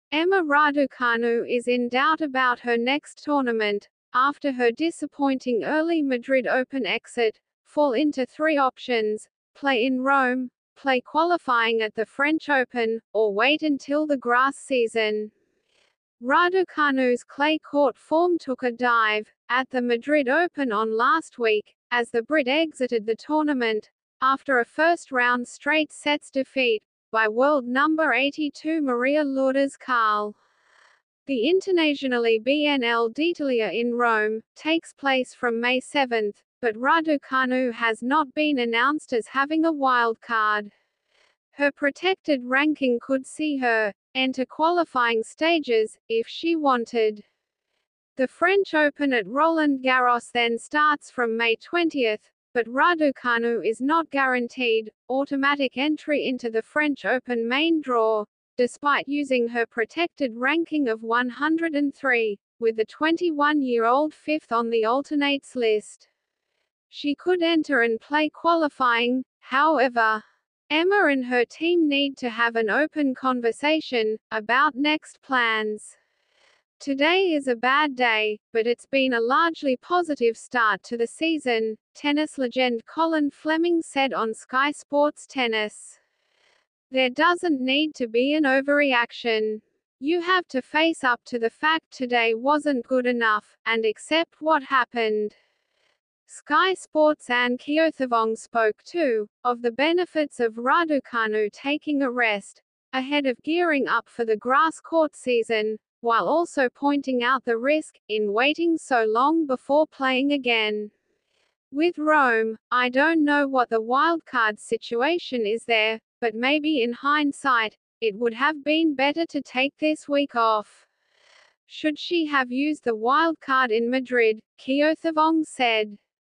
Emma Raducanu's next tournament could be Italian Open or French Open
Emma Raducnu is in doubt about her next tournament after her disappointing early Madrid Open exit fall into three options play in Rome, play qualifying at the French Open, or wait until the grass season.